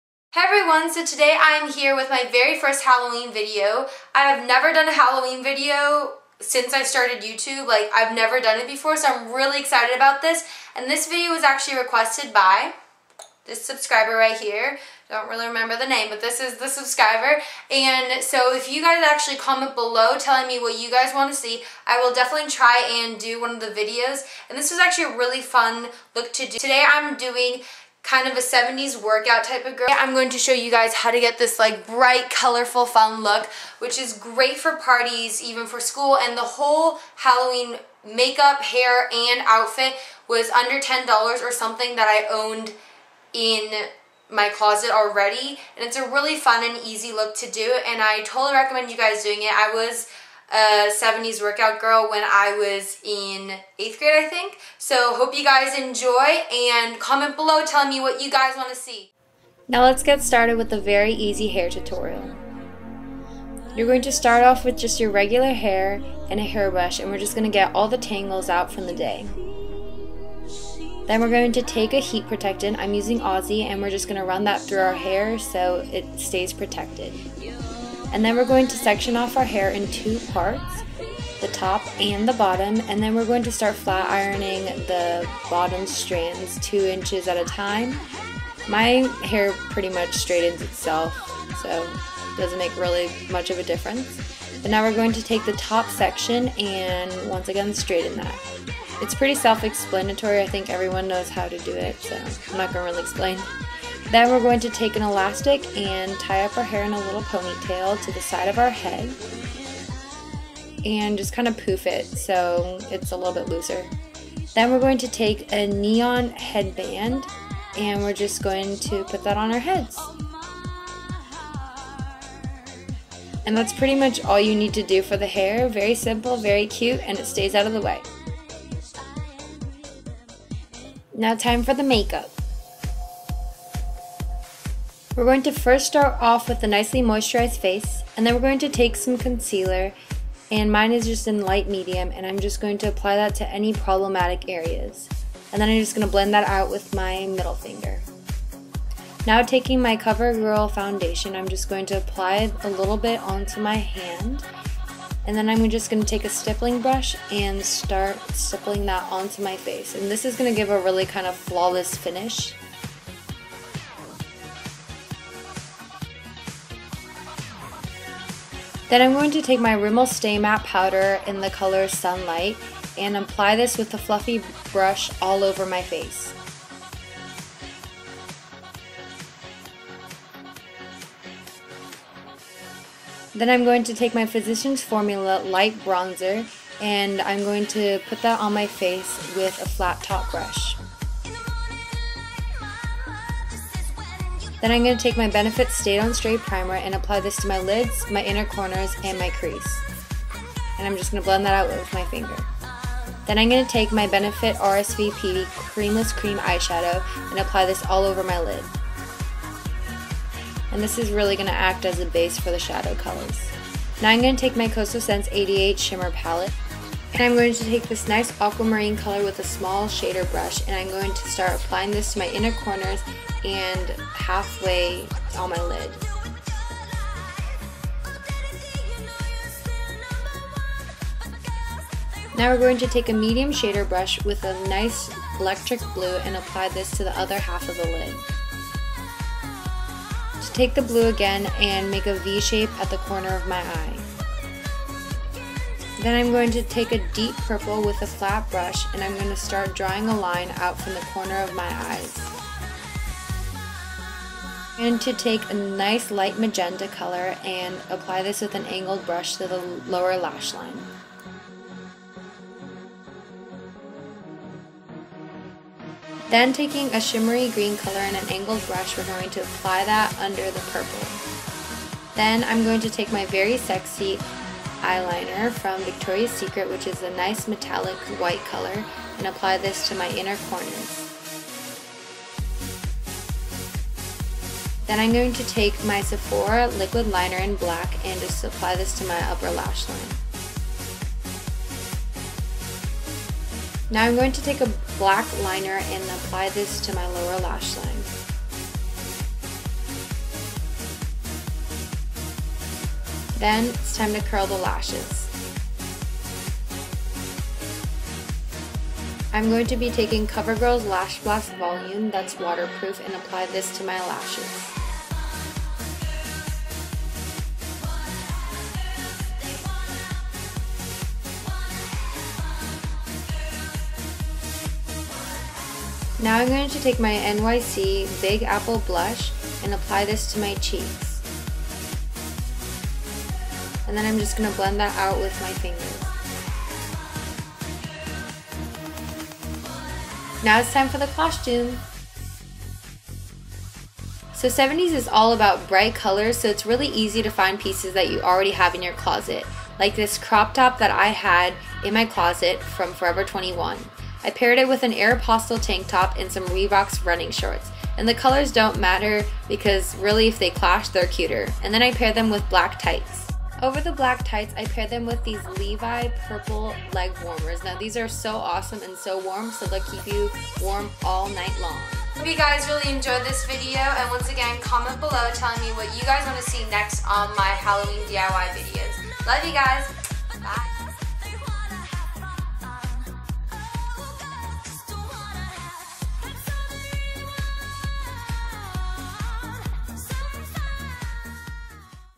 Halloween DIY: 70s Workout girl
hope you guys enjoy! remember, for this look, you can use different colors! i used the ones i did because it matched my top!
please request more halloween costume ideas!
also, big thank you to EVERYONE for getting me to 500 subscribers! love you guys with all of my heart

xoxo-
glitzycouture